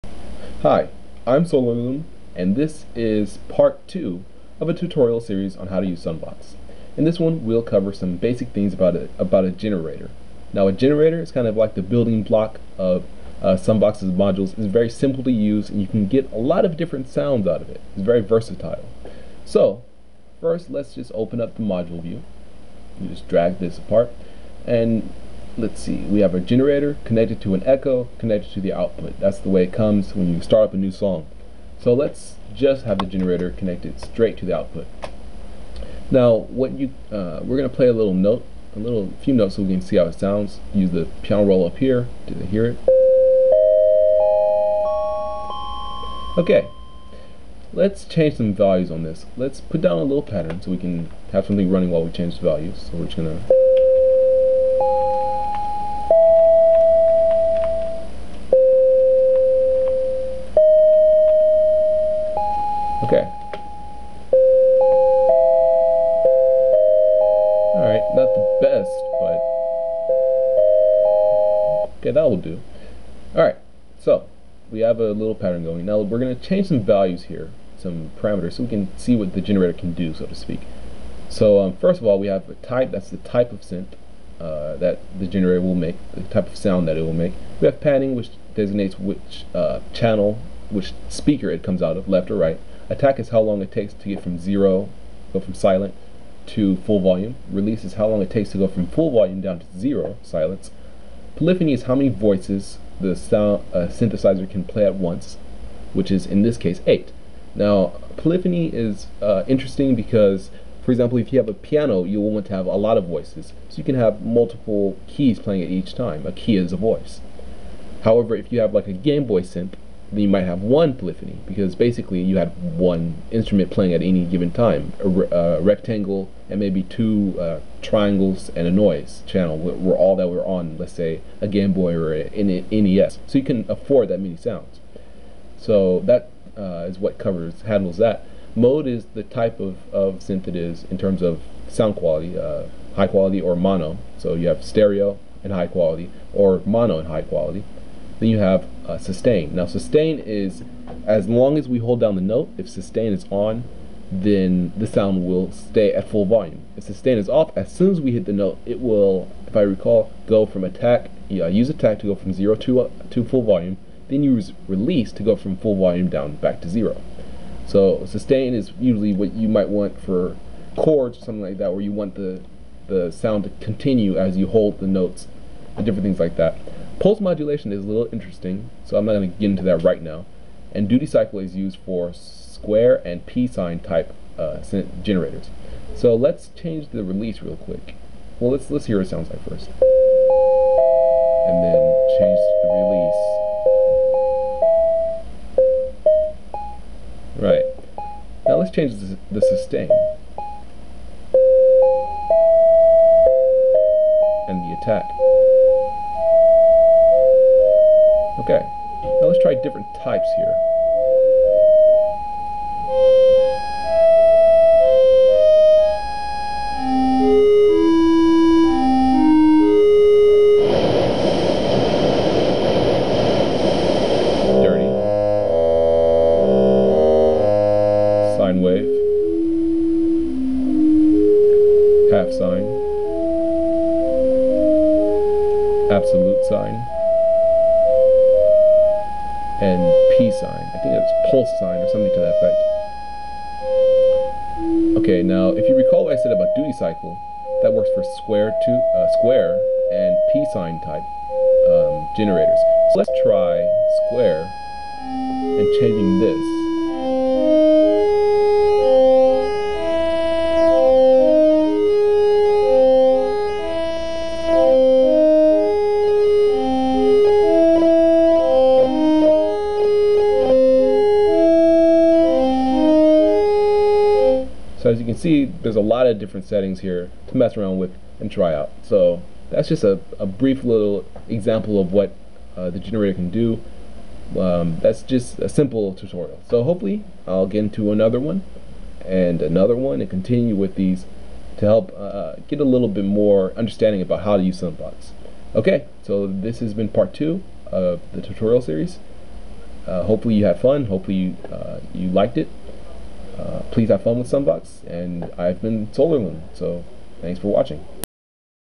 SunVox Tutorial Part 2 - Generator Module
A close look at the generator module, a basic, and yet essential module in SunVox.